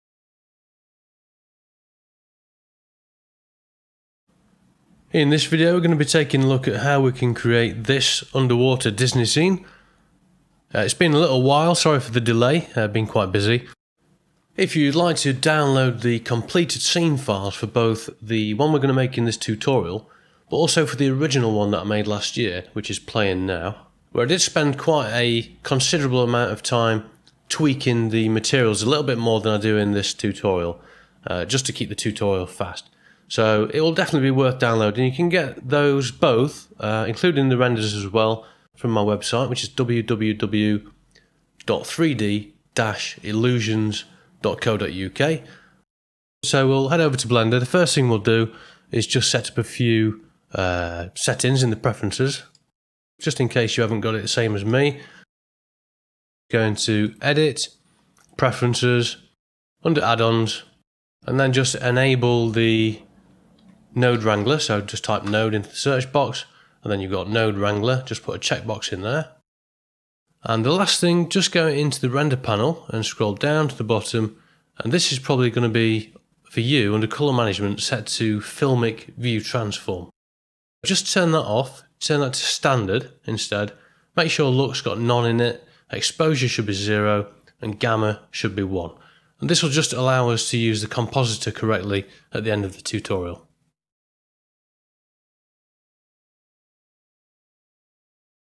Animated Disney Style Underwater Blender Tutorial Part 1
In this 7 part Blender tutorial we'll create a Disney style underwater animation that runs in real-time using the EEVEE render engine.  Other parts currently being processed by Youtube (taking longer than usual)

You can also download the full scene file for both versions, including assets and shaders from:

Topics Covered:

Animated ocean surface (not ocean modifier)
Animated Caustics
Animated Volume Shader
Lighting
Rendering
Modifiers
procedural sand material
Shader Groups
Custom Workspaces
Drivers/Expressions
Graph editor
Colour correction and effects in Blender's compositor.